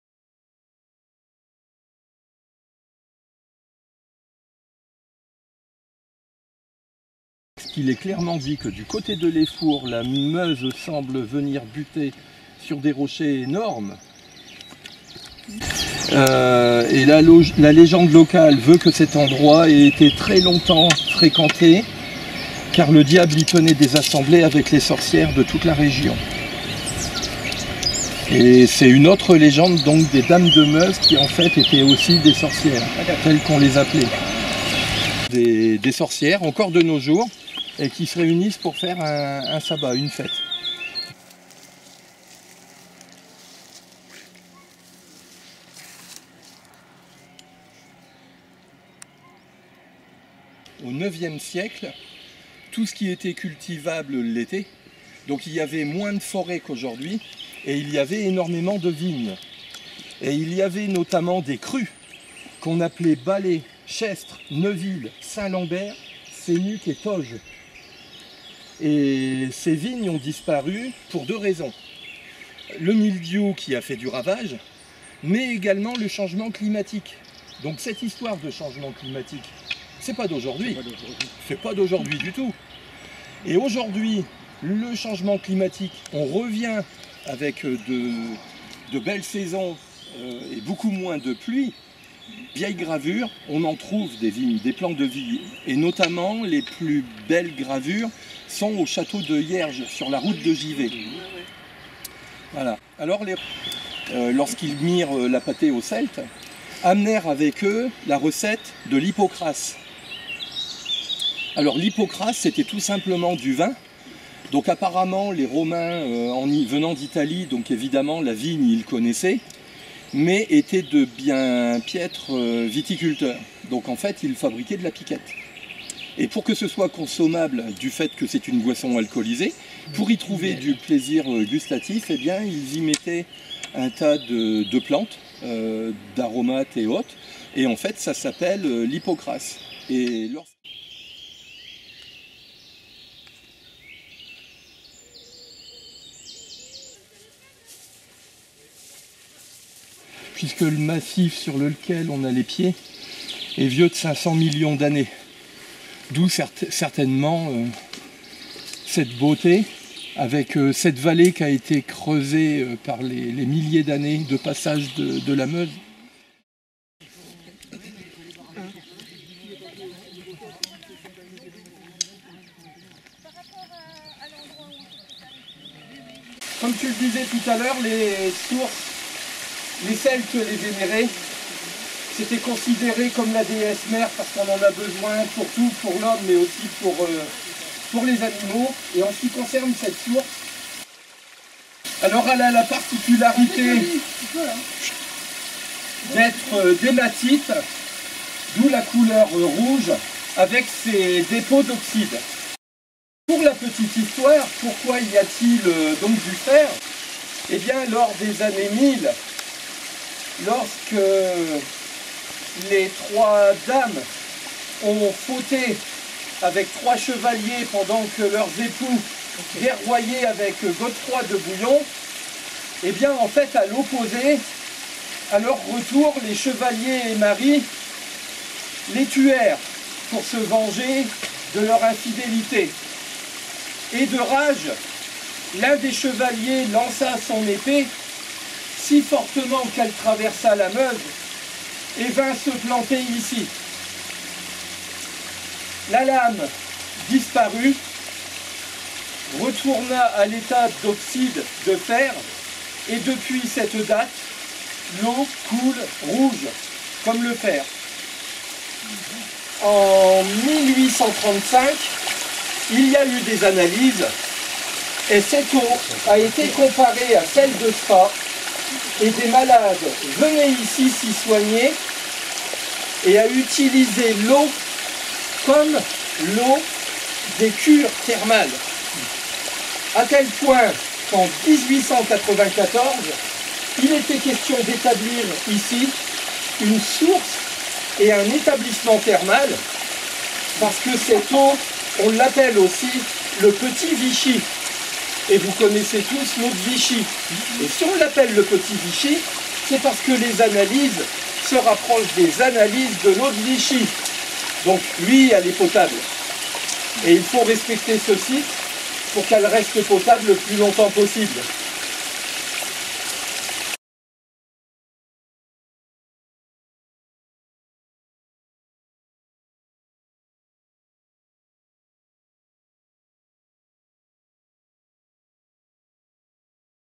LAIFOUR 14 juin
balade au départ de la halte fluviale de Laifour. Guide Jean-Luc Lemaire légendes, nature, histoire sous un beau soleil. communauté de communes Meuse et Semoy.